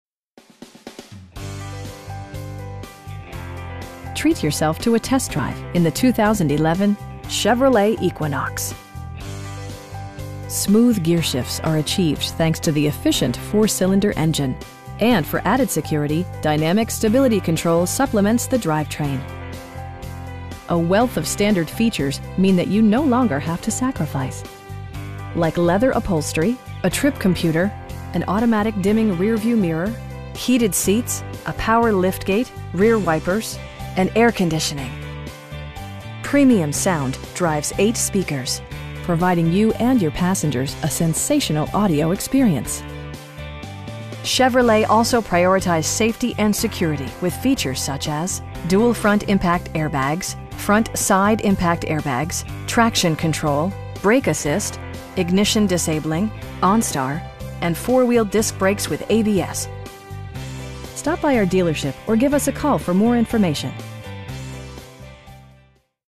2011 Chevrolet Equinox LTZ in Dubuque, IA 52002
Richardson Honda
1475 J.F. Kennedy Rd

Treat yourself to a test drive in the 2011 Chevrolet Equinox. Smooth gearshifts are achieved thanks to the efficient 4 cylinder engine, and for added security, dynamic Stability Control supplements the drivetrain. A wealth of standard features means that you no longer have to sacrifice. Like leather upholstery, a trip computer, an automatic dimming rear-view mirror, heated seats, a power liftgate, cruise control, rear wipers, and air conditioning. Premium sound drives 8 speakers, providing you and your passengers a sensational audio experience. Chevrolet also prioritized safety and security with features such as: dual front impact airbags, front SIDE impact airbags, traction control, brake assist, ignition disabling, onStar, and 4 wheel disc brakes with ABS. Our aim is to provide our customers with the best prices and service at all times. Stop by our dealership or give us a call for more information.